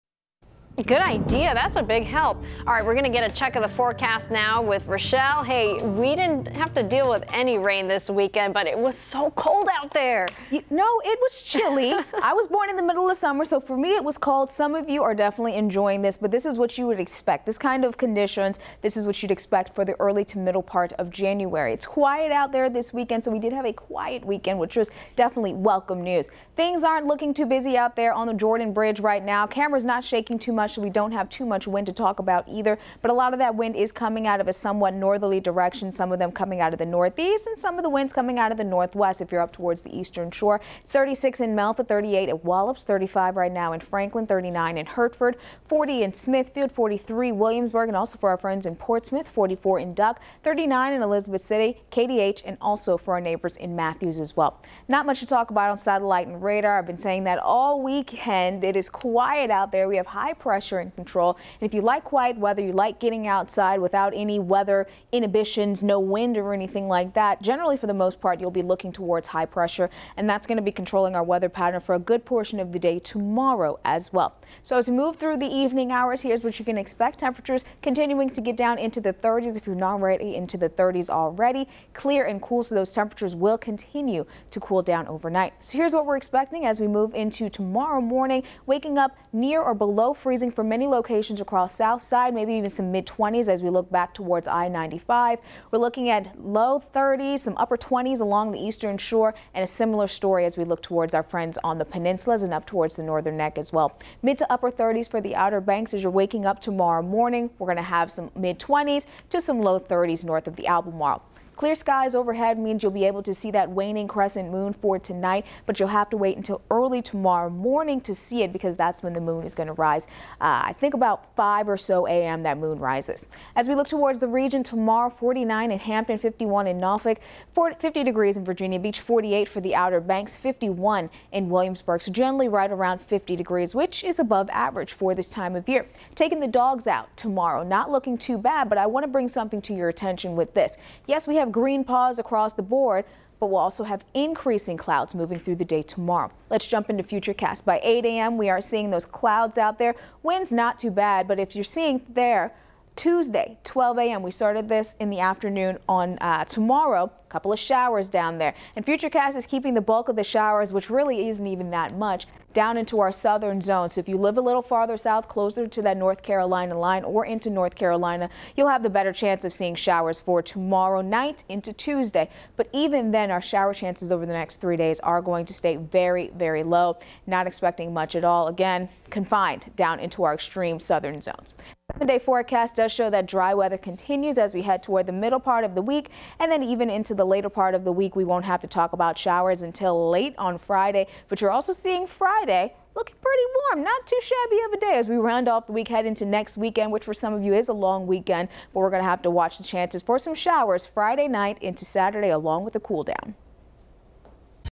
13News Now Weather at 6 p.m. - January 10
Meteorologist Rachael Peart brings you the forecast from the Weather Authority.